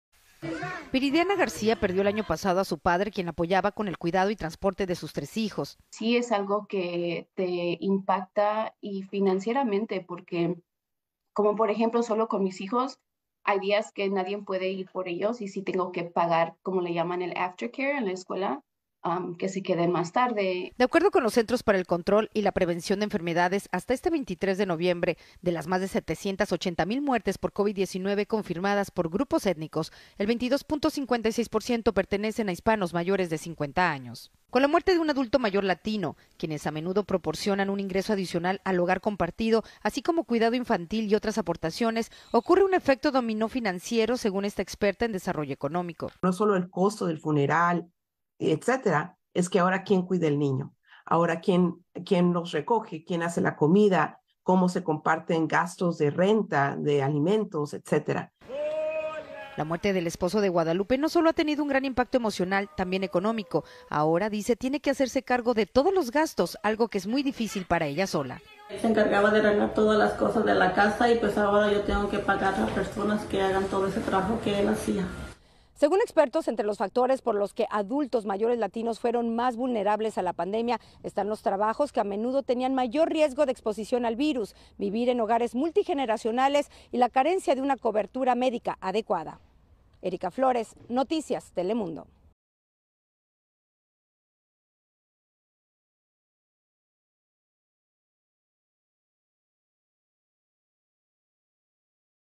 Así las familias latinas han enfrentado las muertes ocasionadas por COVID-19 | Noticias Telemundo
Video oficial de Noticias Telemundo. Más allá del dolor que ha significado perder a un ser querido en la pandemia, hay quienes por su ausencia ahora tienen que lidiar con problemas económicos y nuevas dinámicas familiares.